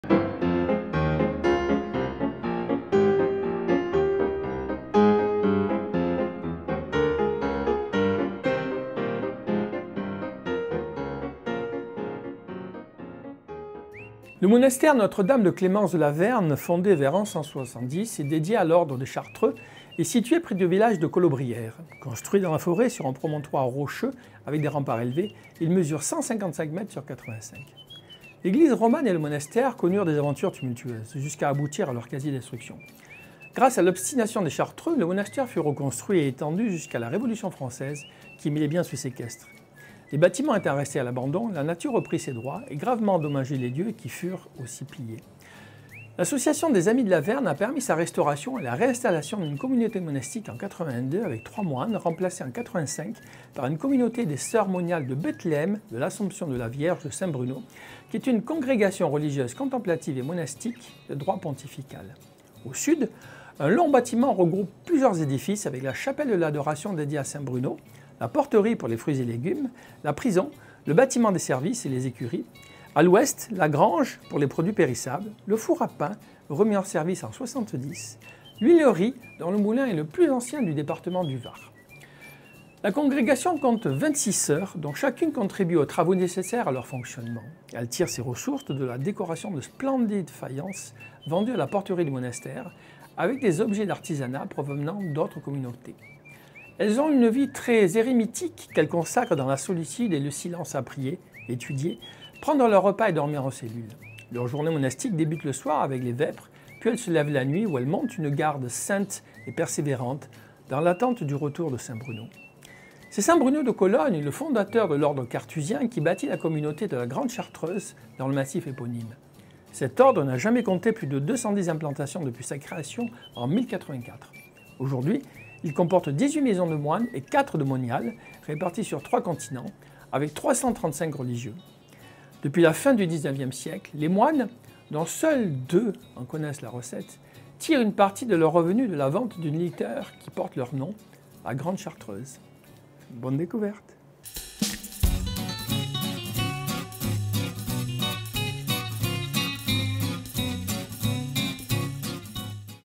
CHRONIQUES-CULTURE / Les escapades de Jean-Pierre / La Chartreuse de la Verne
Pour cette nouvelle escapade, Jean-Pierre nous emmène près de Collobrières, dans le Var, visiter le monastère Notre-Dame de Clémence de la Verne dédié à l'Ordre des Chartreux. Bonne découverte à tous !